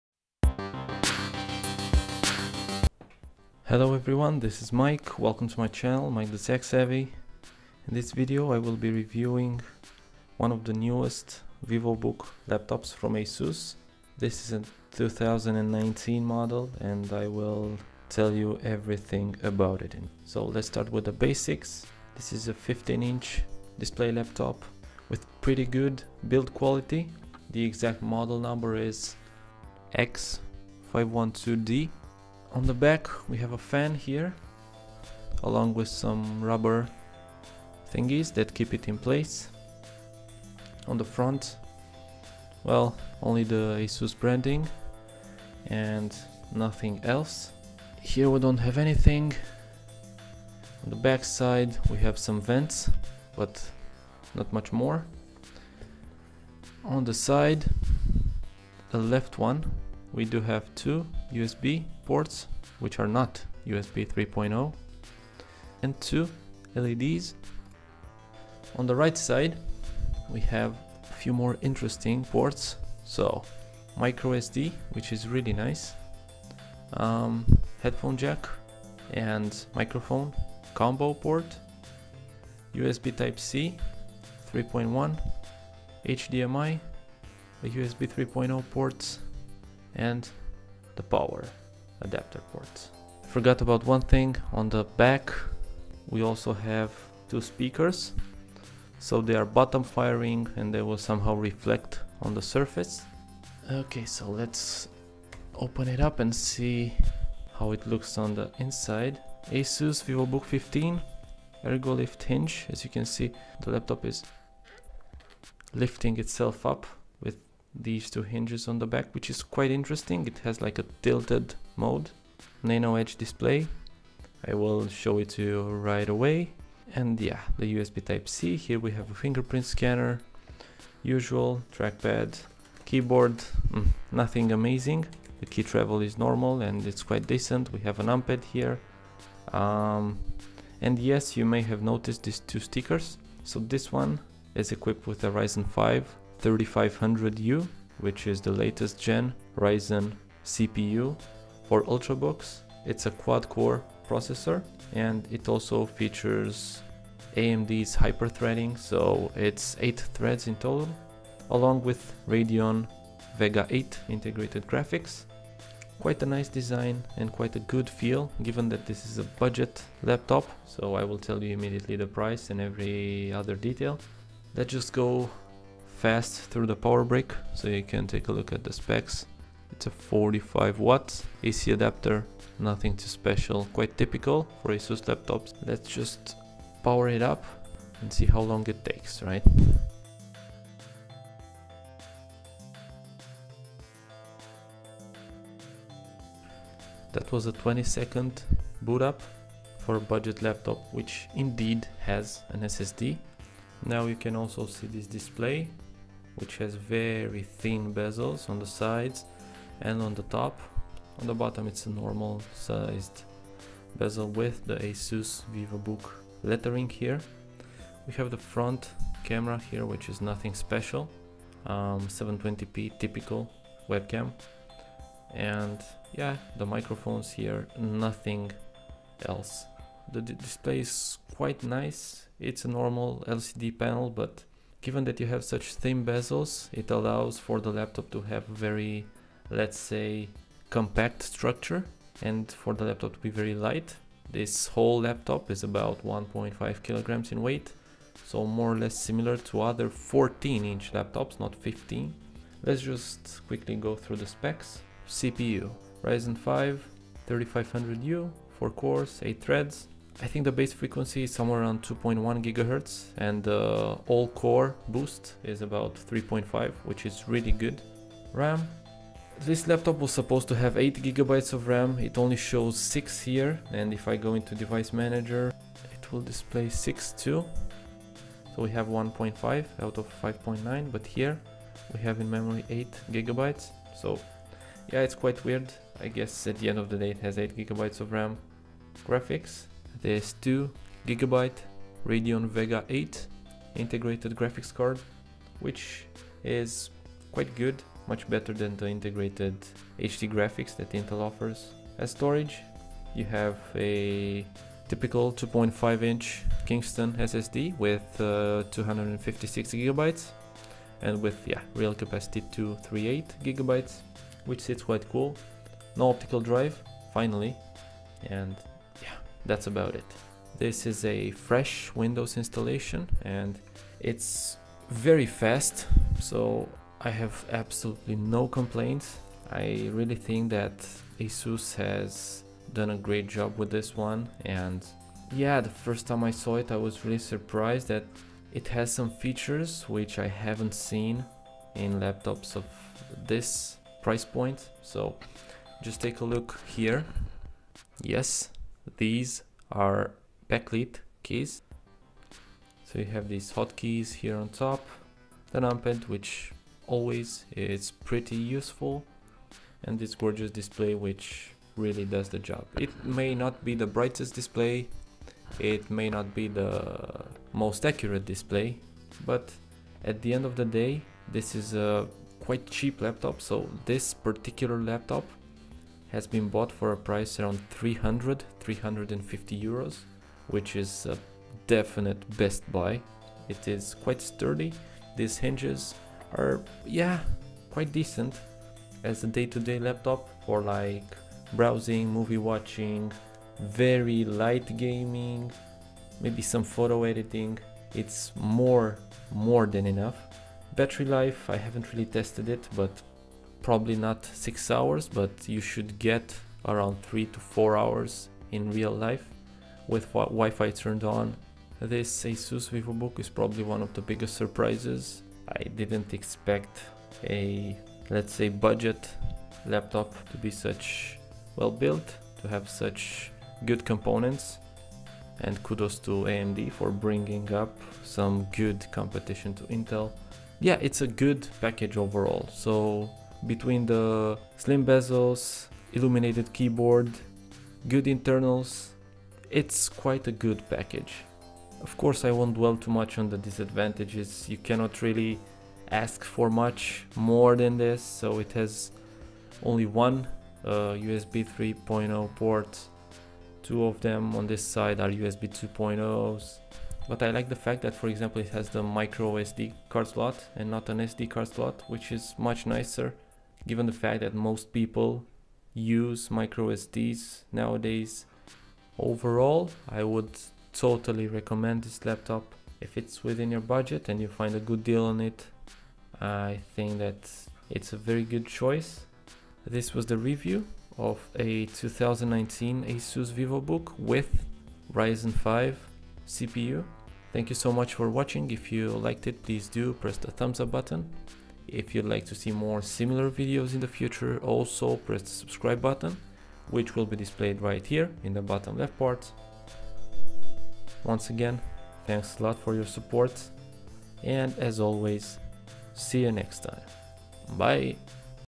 Asus Vivobook 15 - In-Depth Review - Best Budget Laptop for 2020?
Here's a full in-depth review of the budget Asus Vivobook 15 laptop (X512D).

The main specs of the laptop I've tested are: AMD Ryzen 5 3500U, 15" Full HD LCD display, 8GB RAM, 256GB SSD.